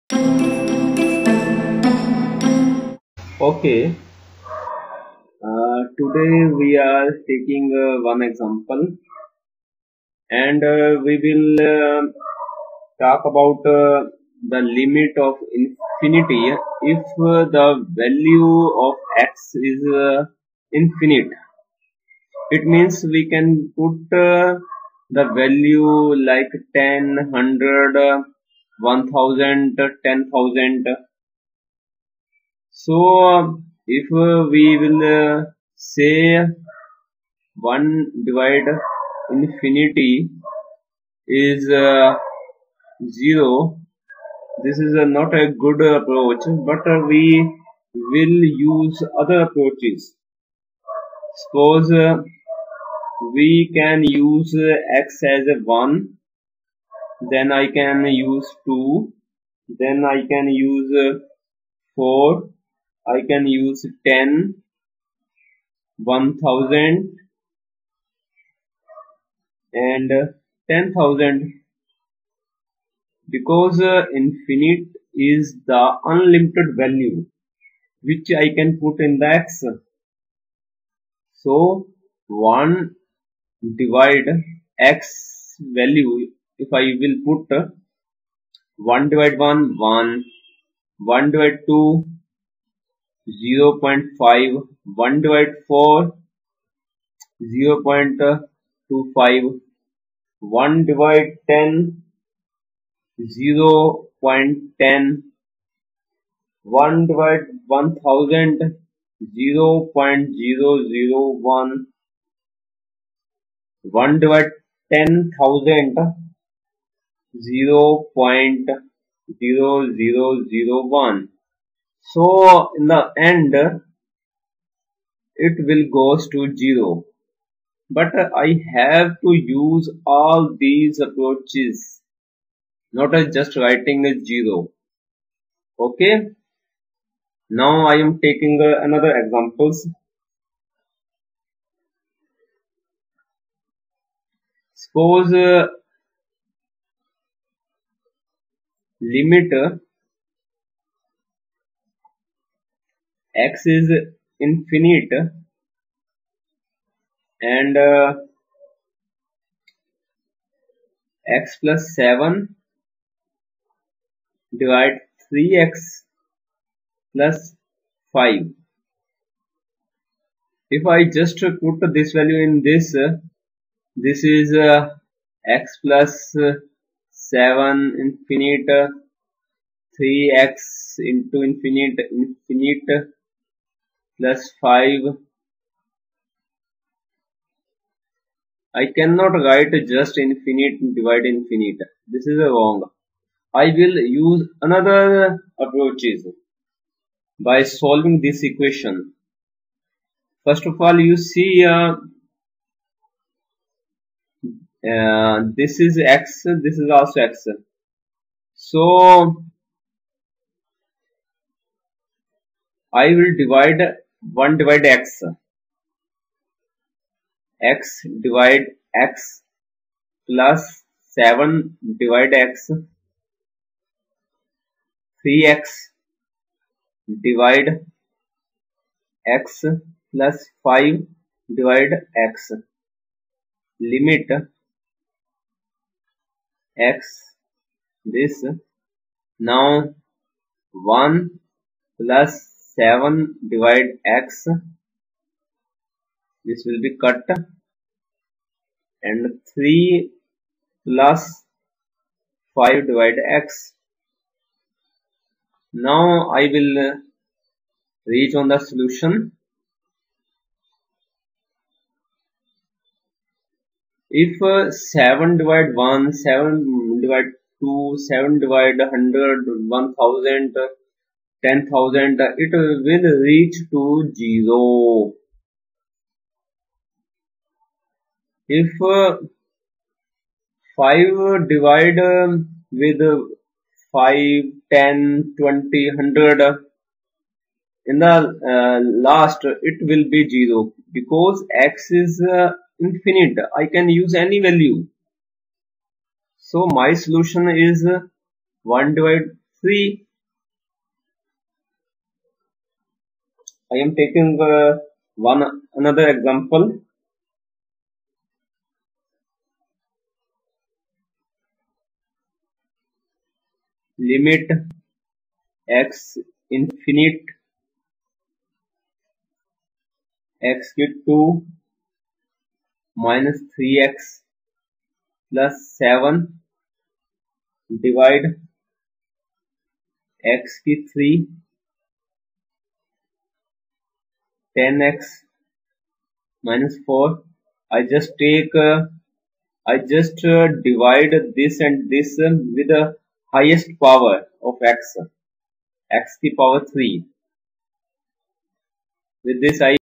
Evaluate Limit to Infinity
Infinity is unlimited value which we can use. So, for evaluation limit to infinity in the any function formula, we can use limitless approaches for reaching best solution.